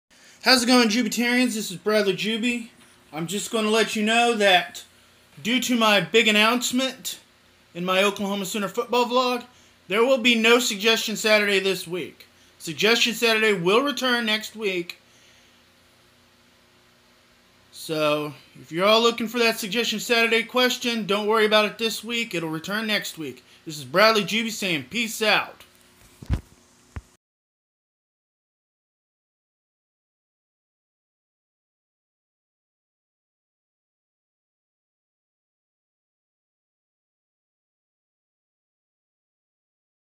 No Suggestion Saturday This Week!
Due to my "Big Announcement," there will not be a Suggestion Saturday this week.